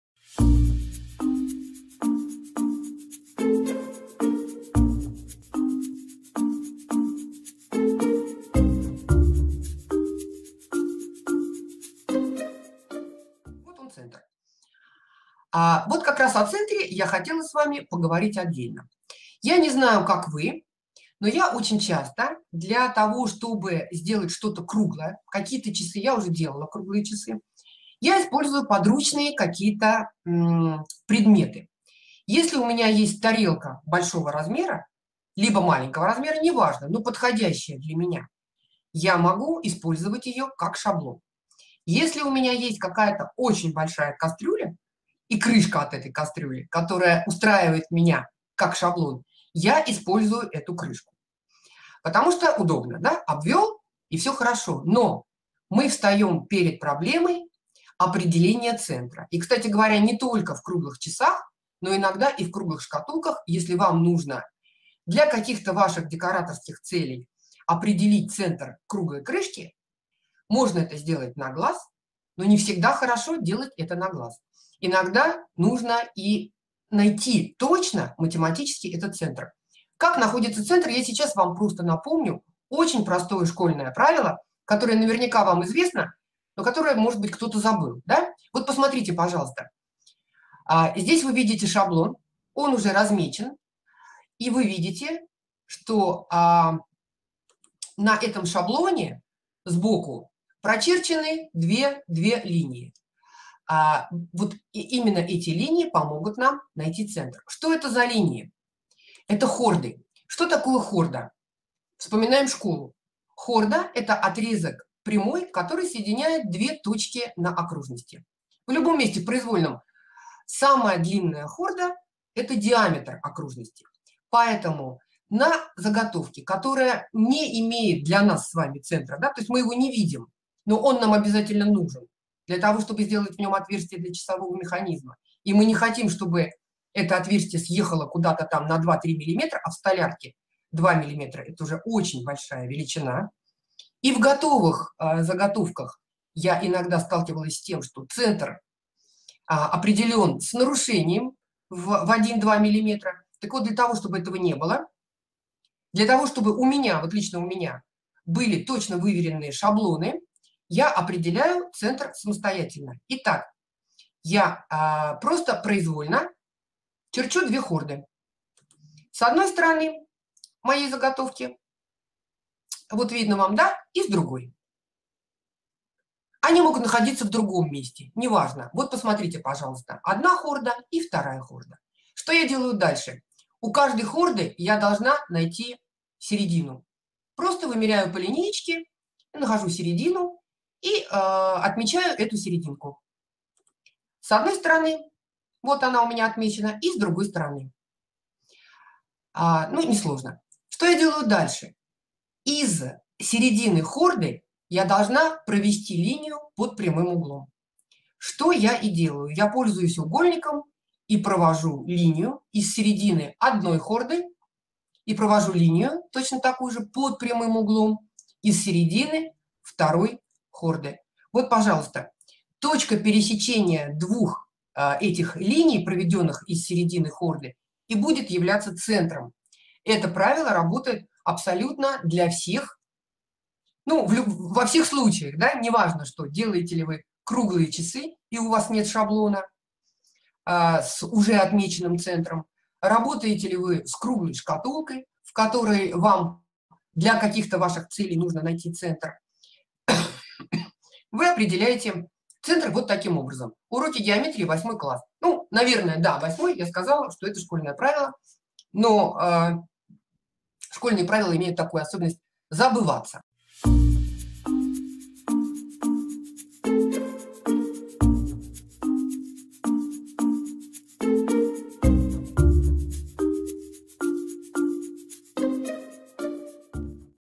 Определение центра окружности на часах. Университет декупажа. Екатерина Васильева
Всего за 5 минут Екатерина расскажет как легко и просто определить центр окружности на часах:
Подробная и легкая инструкция от мастера!
Что такое хорда?А вы помните?